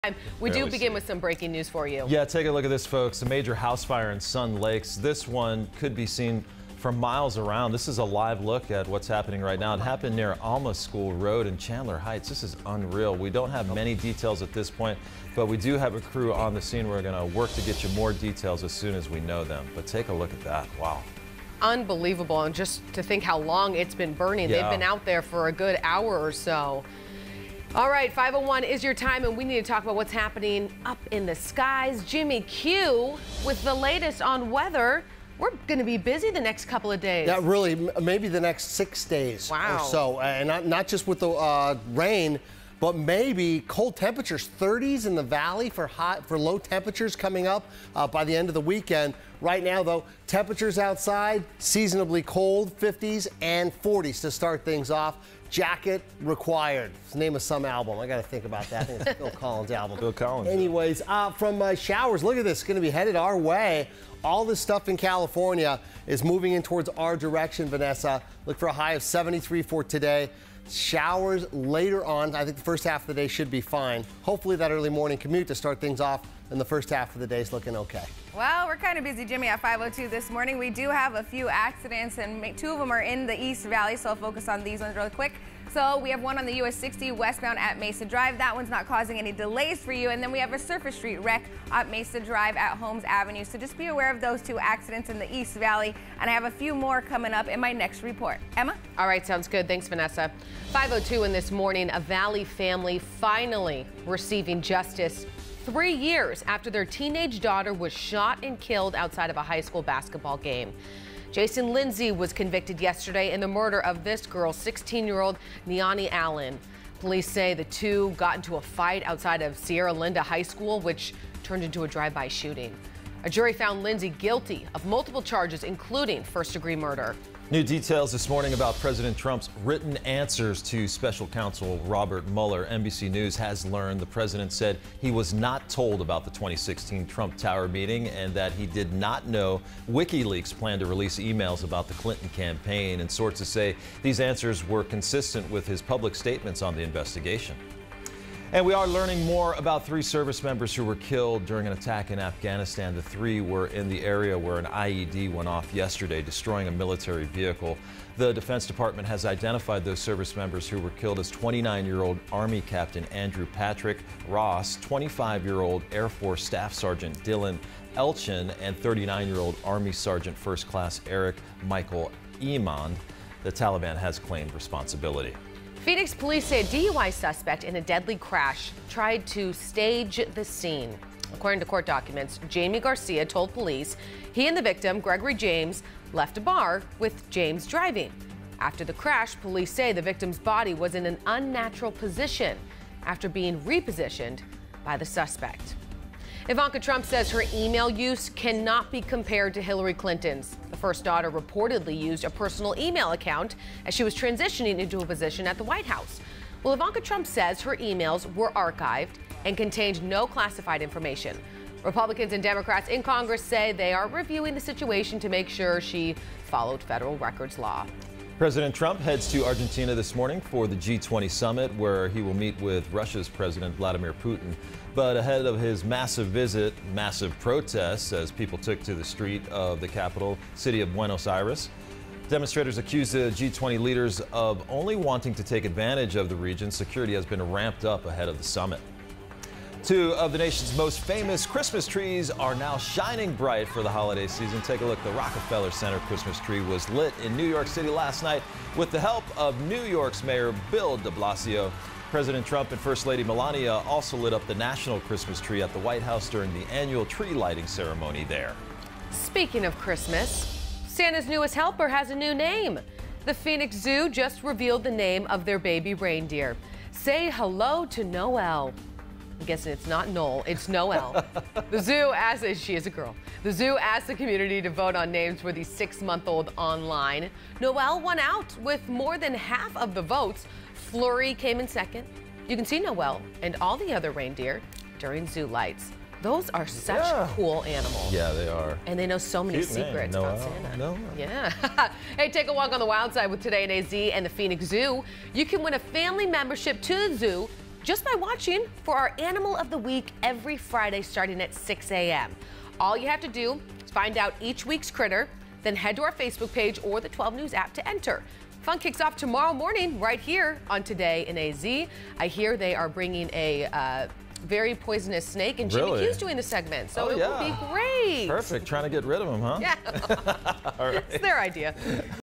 Thursday's Up to Speed on Today in AZ
Major house fire in Sun Lakes, Weather, Traffic, teen murder verdict, Trump vs. Mueller, three service member killed in Afghanistan, DUI suspect tried to stage scene, Ivanka Trump's email, President Trump heads to Argentina, famous Christmas tree lightings, and the name Noelle wins for new baby reindeer.